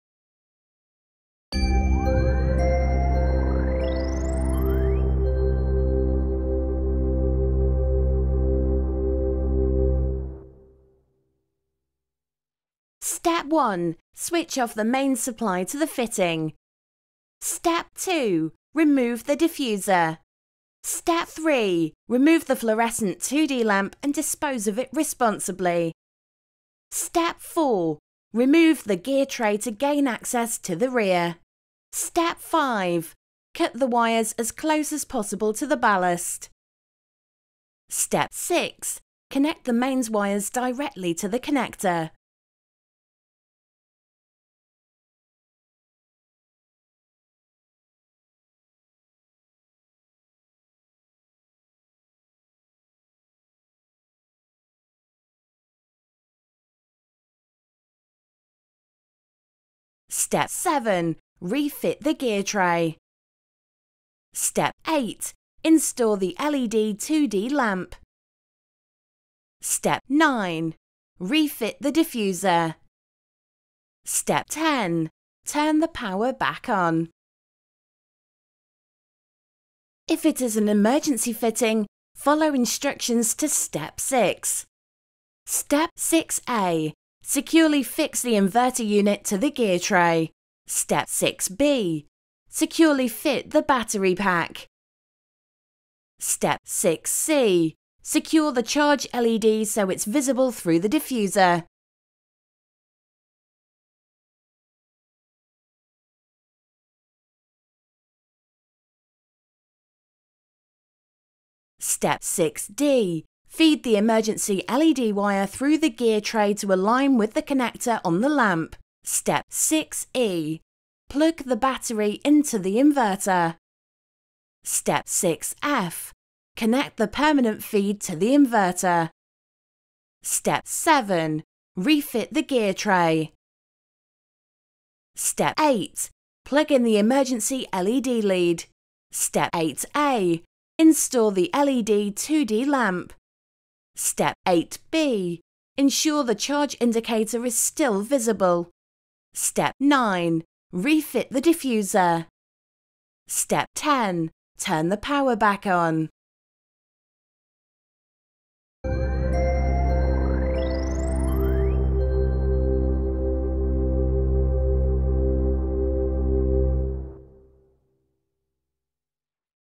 How to install an Earlsmann LED Replacement 2D lamp
If you ever wanted to know how to install an Earlsmann LED replacement 2D lamp, including the emergency version, watch this short film.  These 12W LED lamps are low carbon, low energy, replacements for the standard 28W cfl type